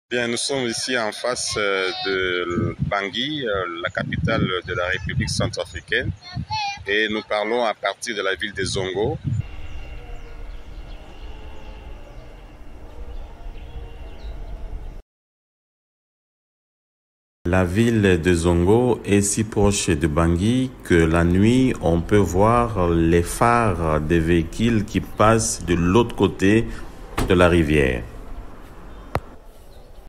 Zongo (RDC) juste en face de Bangui (RCA)
La ville congolaise de Zongo (dans la Province du Sud-Ubangi) est située juste en face de Bangui (la capitale de la République Centrafricaine). Les deux pays sont séparés par la rivière Ubangi.
Nous avons voulu vous en donner un aperçu !